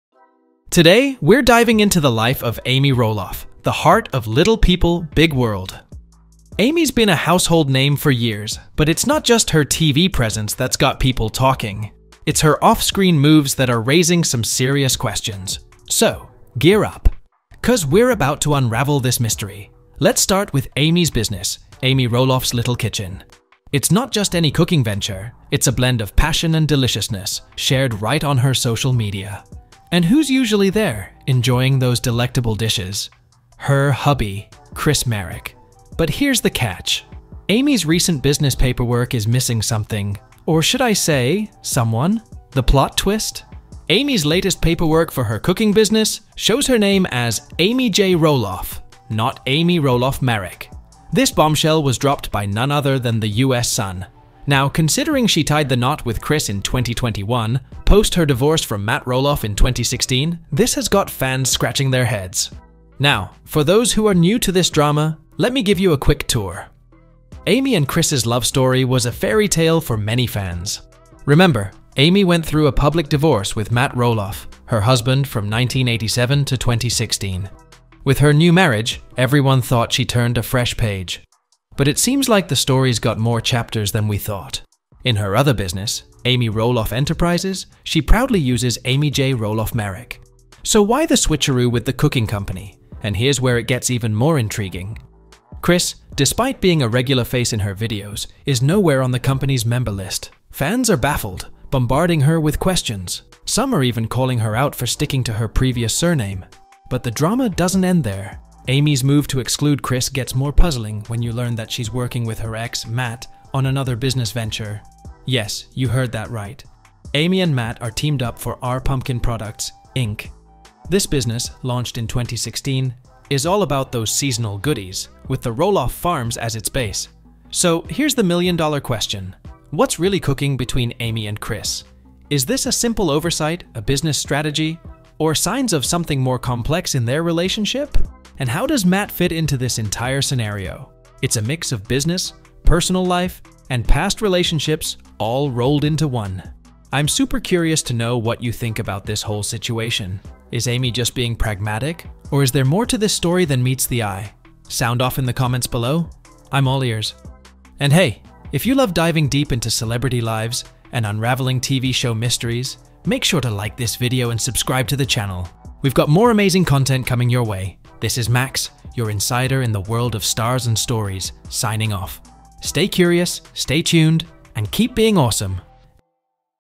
Amy Roloff snubs husband Chris Marek, marriage is in trouble - LPBW Family
Today, we're diving into the life of Amy Roloff, the heart of "Little People, Big World." Amy's been a household name for years, but it's not just her TV presence that's got people talking. It’s her off-screen moves that are raising some serious questions. So, gear up, 'cause we're about to unravel this mystery.

Let's start with Amy's business, Amy Roloff’s Little Kitchen. It's not just any cooking venture; it's a blend of passion and deliciousness, shared right on her social media. And who's usually there, enjoying those delectable dishes? Her hubby, Chris Marek. But here's the catch – Amy's recent business paperwork is missing something... or should I say, someone?

The plot twist? Amy’s latest paperwork for her cooking business shows her name as Amy J Roloff, not Amy Roloff-Marek. This bombshell was dropped by none other than The U.S. Sun. Now, considering she tied the knot with Chris in 2021, post her divorce from Matt Roloff in 2016, this has got fans scratching their heads.

Now, for those who are new to this drama, let me give you a quick tour. Amy and Chris’s love story was a fairy tale for many fans. Remember, Amy went through a public divorce with Matt Roloff, her husband from 1987 to 2016. With her new marriage, everyone thought she turned a fresh page. But it seems like the story's got more chapters than we thought.